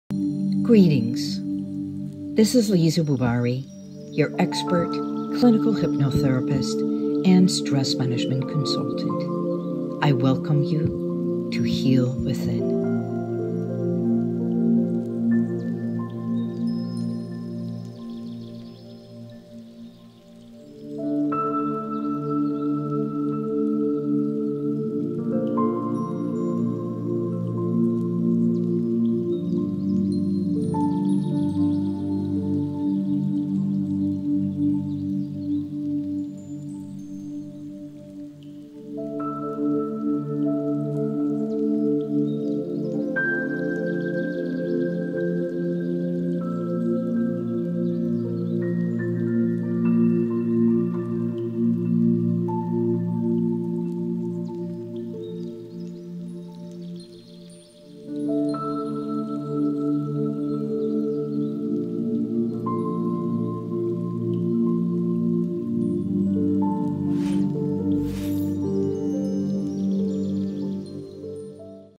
HealWithin offers you Hypnotherapy and Stress Management
I help women heal emotional pain and gain renewed confidence through hypnotherapy, stress management, and action coaching.
It is time to Evoke, Embrace & Evolve to the best version of you.
You can start by booking a free consultation today